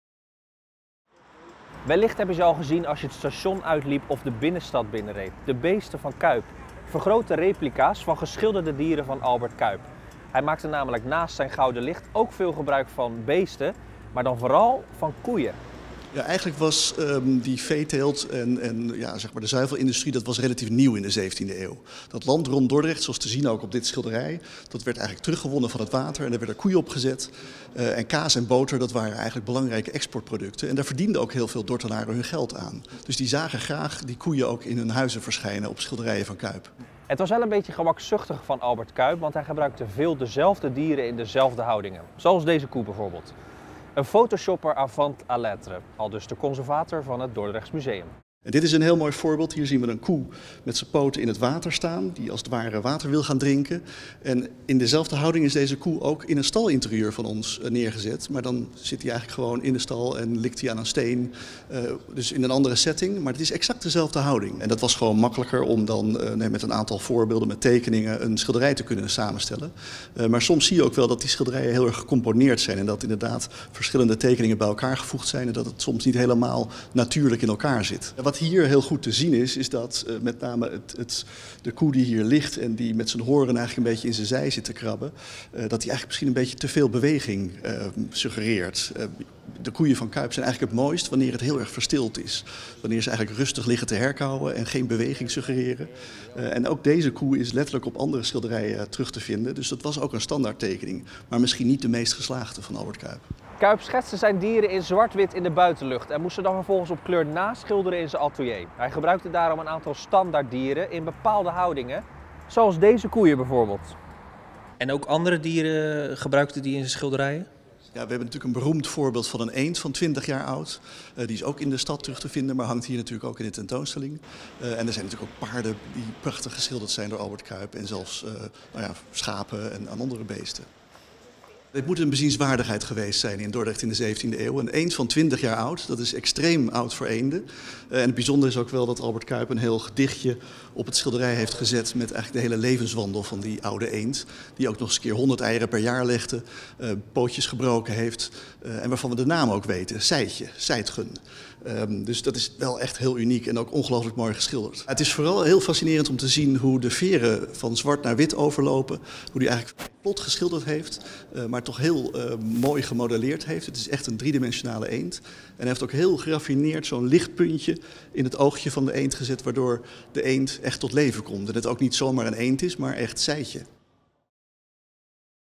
Cuyp schilderde vooral veel koeien, maar kopieerde ze ook vaak
Albert Cuyp wordt niet alleen geroemd om zijn ‘gouden licht’, maar ook door het gebruik van beesten. Vooral koeien waren zijn favoriet. De Dordtse kunstenaar schilderde zijn koeien als ‘waardige dieren’, maar tegelijkertijd was hij ook een beetje een photoshopper avant la lettre: “De exact zelfde houding in een andere setting op een ander schilderij.”

"De veeteelt en zuivelindustrie waren relatief nieuw in de 17e eeuw. Kaas en boter waren belangrijke export producten,” verklaart Sander Paarlberg, conservator van het Dordrechts Museum. Er werd veel geld aan de koeien verdiend en dus zag de Dordtse elite deze geschilderde koeien graag in hun huis hangen.

Meer lezen?